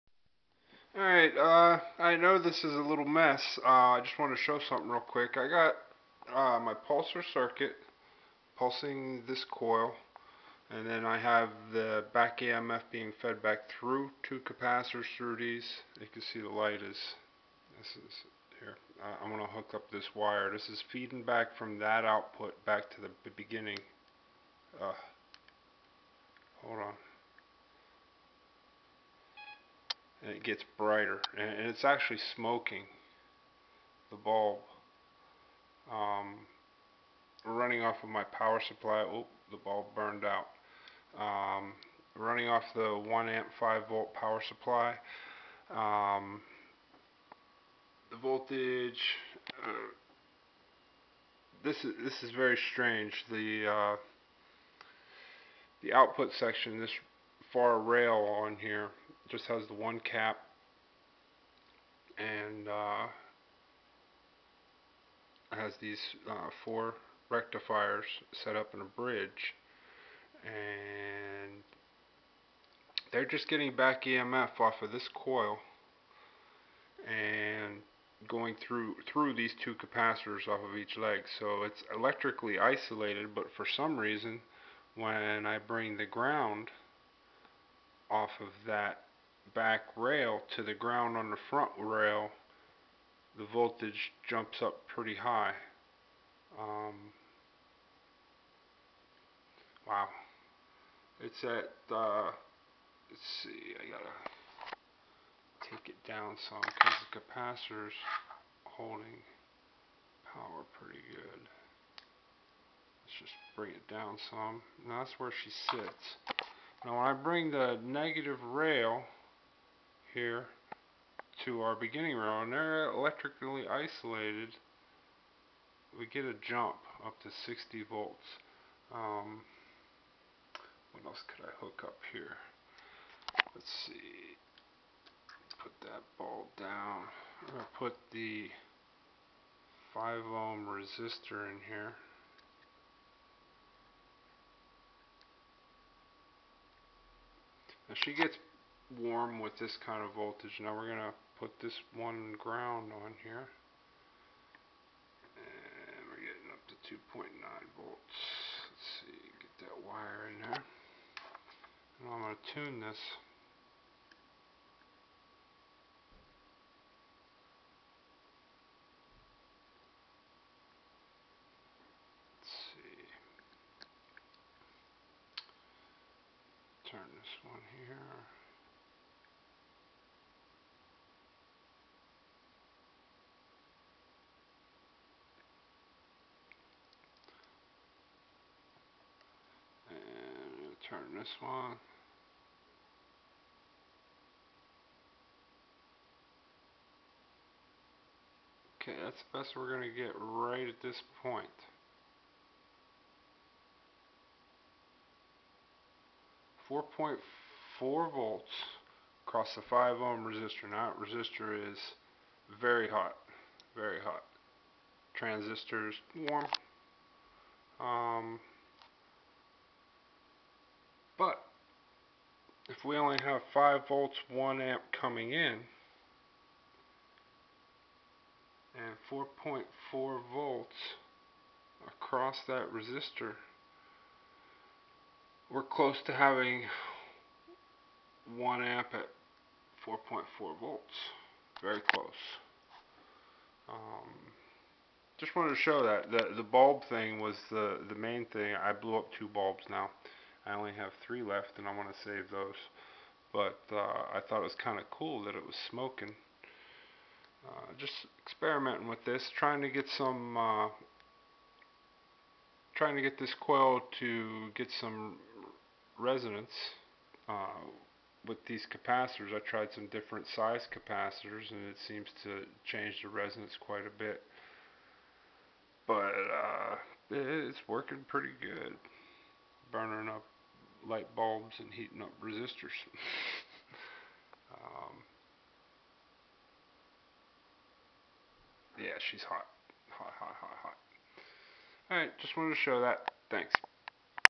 Big coil, Back EMF, and Smoking Light Bulbs, And some errors
In this vid, I made an error in saying that the back emf capture circuit was isolated from the pulse input circuit by way of the bemf being put through the 2 blue caps. After I realized that I forgot that I had switched the caps to be in parallel with each other on 1 lead coming from the coil to increase the capacitance while the other lead of the coil was direct to the bridge. Before this vid I had the caps 1 on each lead, thereby I had true isolation. After watching the vid, the part where I connected the neg rails of the output to the input, you see the light go out. So the neg rail shorted to the pos, and the light went out for a sec. I didnt realize that when it happened.
But I had another error here that I caught also. I had the diodes that form the bridge reversed. Notice my output on the meter and where my meter is connected on the board. This would cause, what I thought was the neg output rail, to be pos. Thus when I connected what I thought was neg out to the input neg, light went out because of short on the PS.  I thought man Im a loon.
But there was still a significant increase in output, 30v to 60v by hooking one of the outputs to the input.
Now 5v in, added to the 30v out will not make 60v. I have to revisit this circuit, whitch is basically a switching supply, chopper, because this effect, now after a week, has my attn. beyond the errors.  A normal chopper will not use through caps on the output and also not normally have the input tied to the output in any way.
So my next vid will show results from a corrected bridge and the difference between the in and out connections effects.  These errors are why I had not posted the vid, but now it has me thinking.